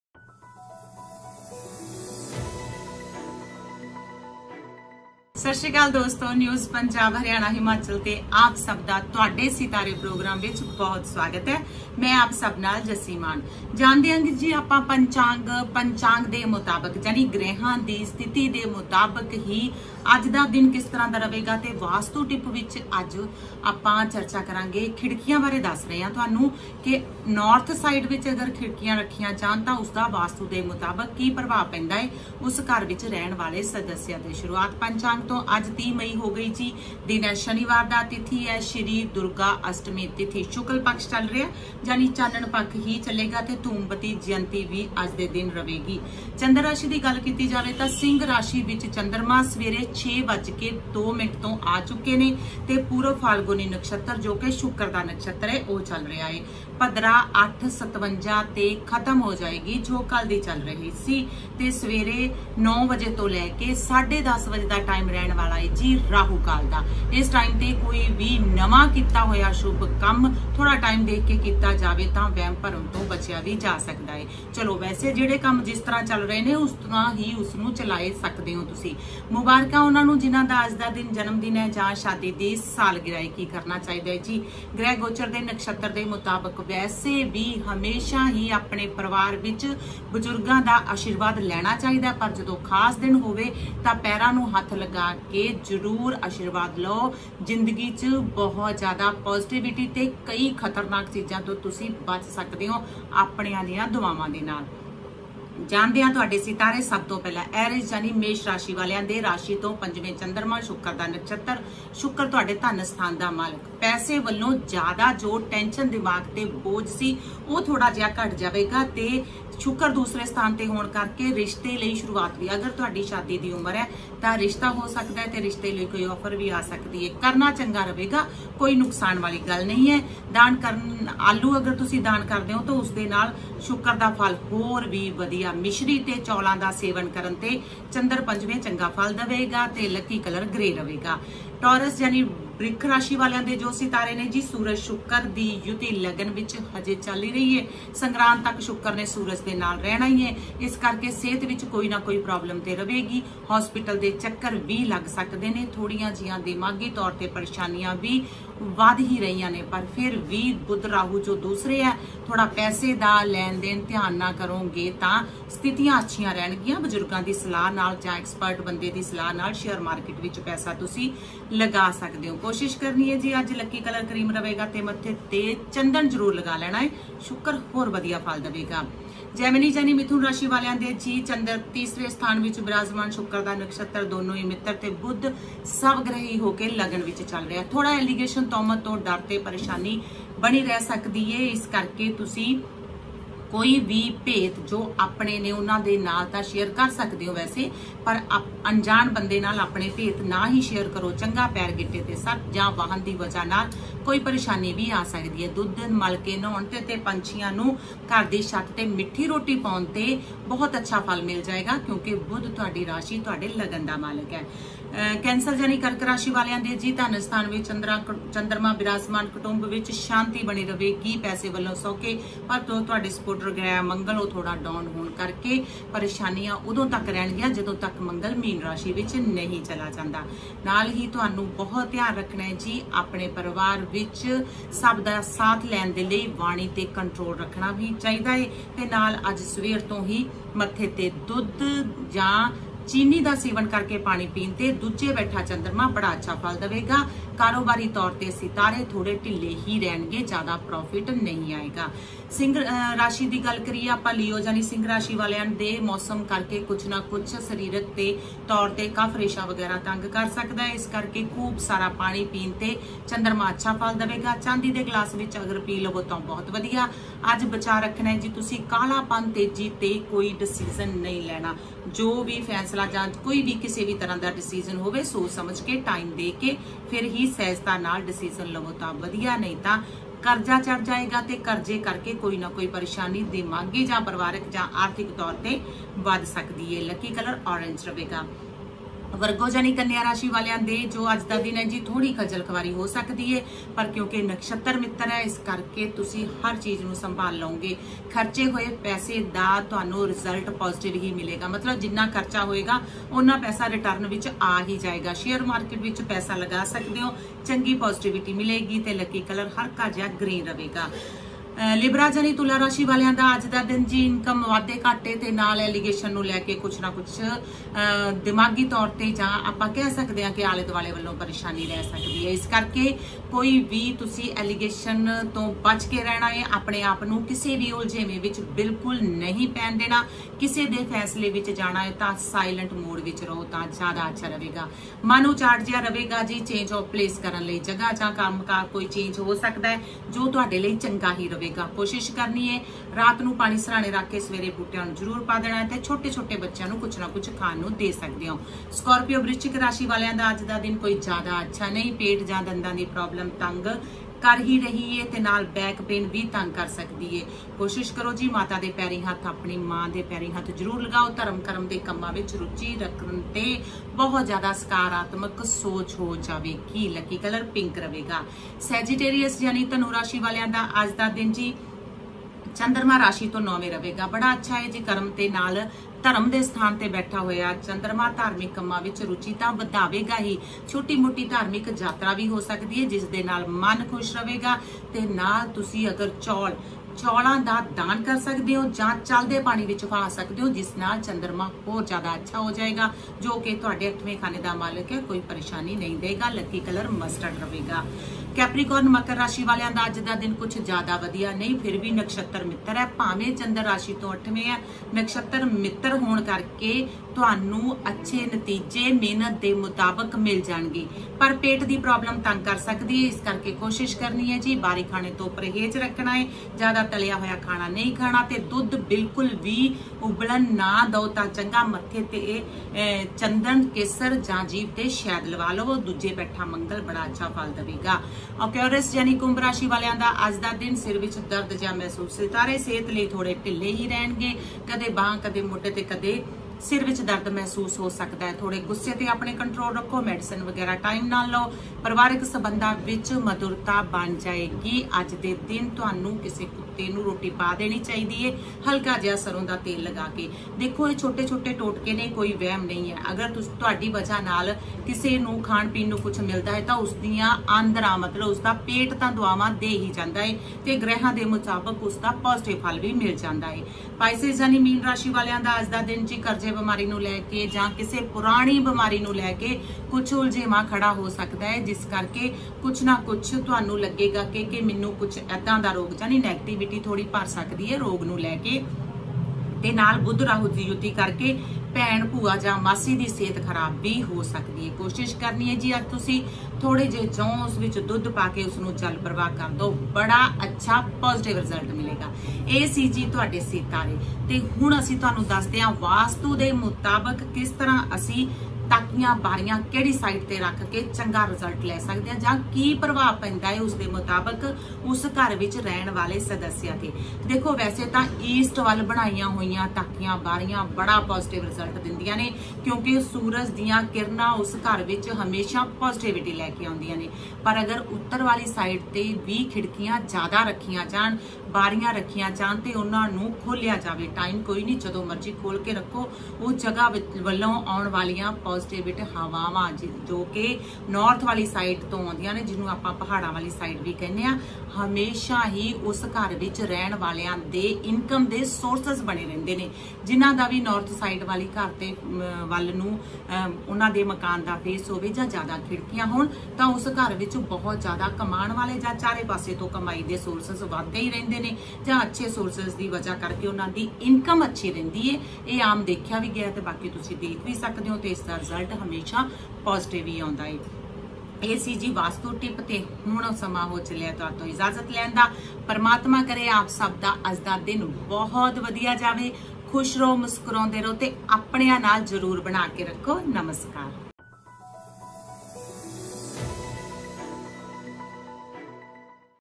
क्या कहते हैं ‘ਤੁਹਾਡੇ ਸਿਤਾਰੇ’
कैसी है आपके सितारों की चाल, आज आपके लिए क्या रहेगा शुभ, जानिए जस्सी मान के साथ सिर्फ NewsPHH पर, क्या कहते हैं ‘ਤੁਹਾਡੇ ਸਿਤਾਰੇ’